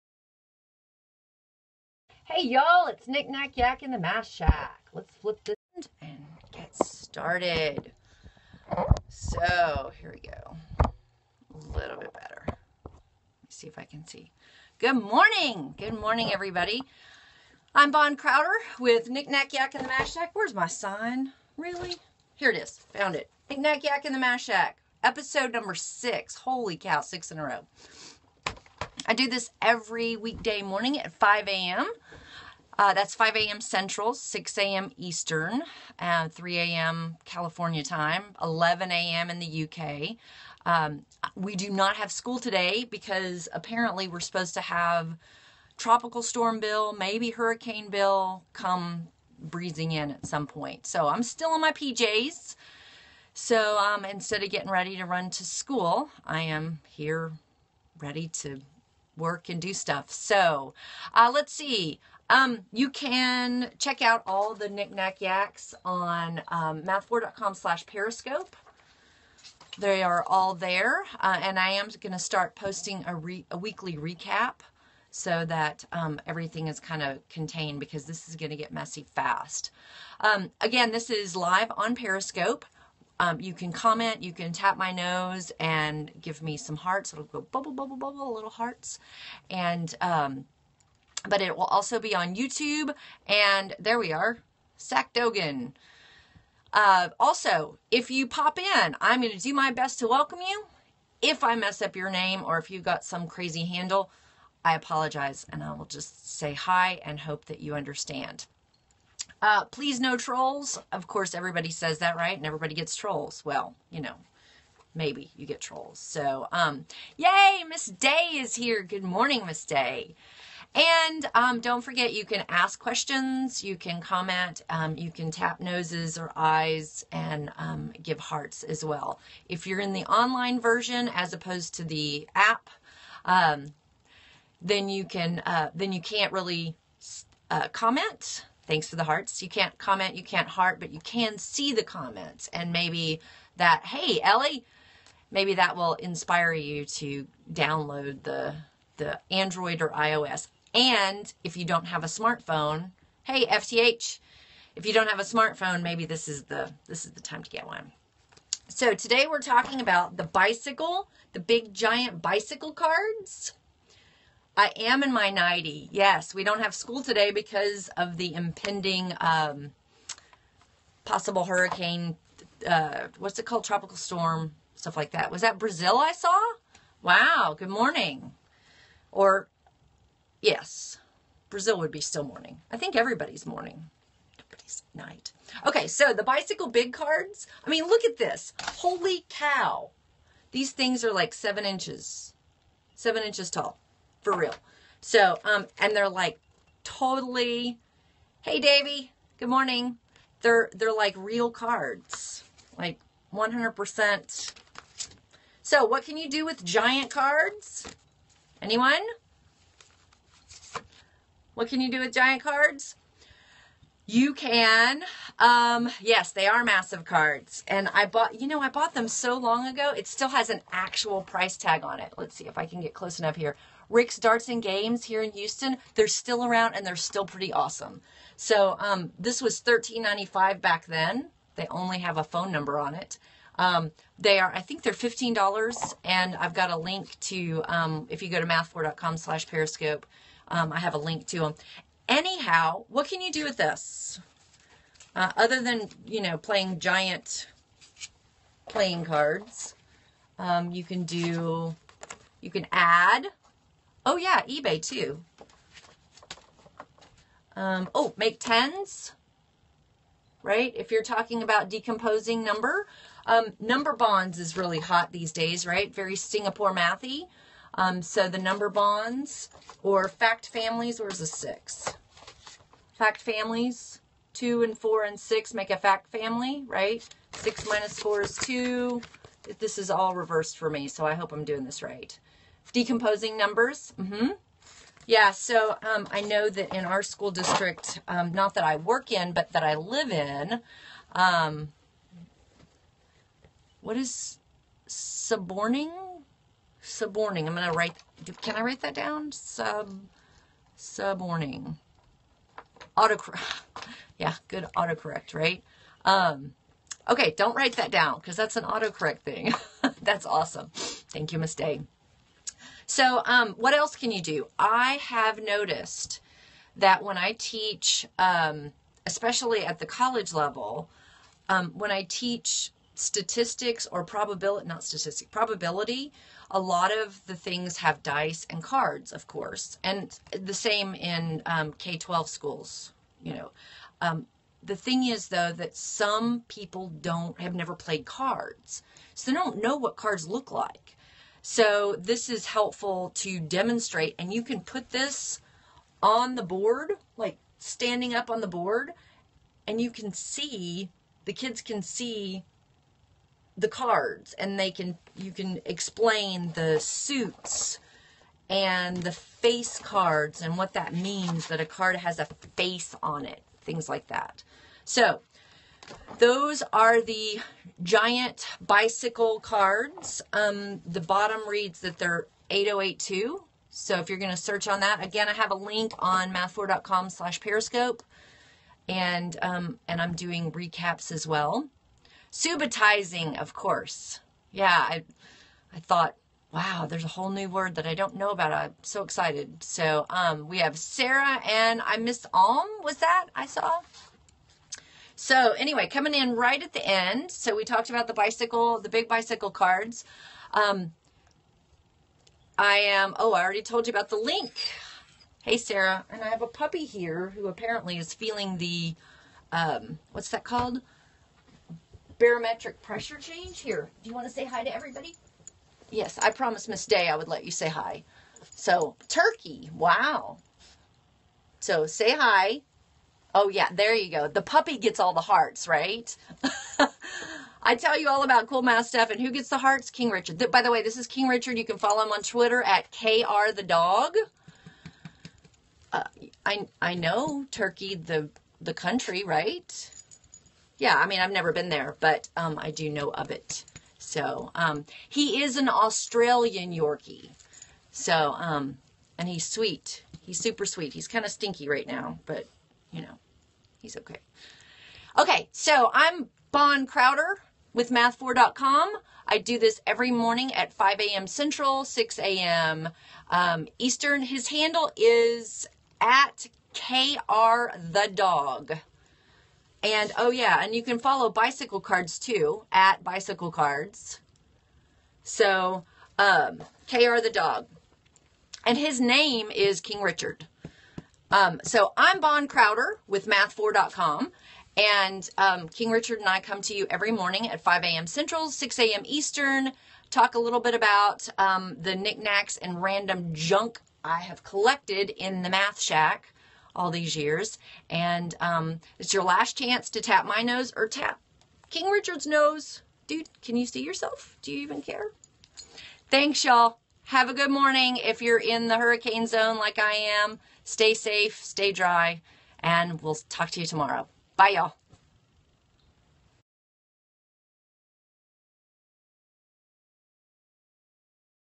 Knick Knack Yack in the MathShack - BIG Bicycle Playing Cards
From the MathShack in Houston, Texas, Bon Crowder shows all the knick-knacks and random math stuff that she's collected over the years.

Today's episode is about my BIG Bicycle playing cards - a great tool for classrooms. It's good for decomposing numbers (number bonds), math facts and teaching probability.